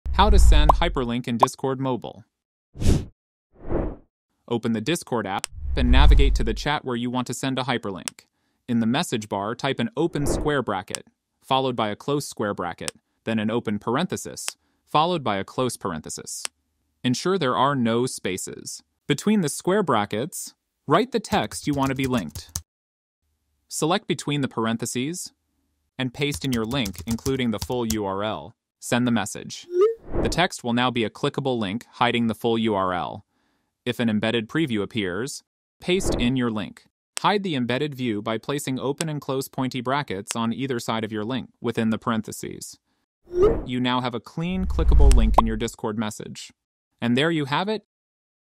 How To Send Hyperlink In Discord on Mobile?
Learn how to easily send hyperlinks in Discord on your mobile device in this step-by-step tutorial. With the latest updates in 2024, the process has become even simpler. Follow along to share links with your friends and community effortlessly.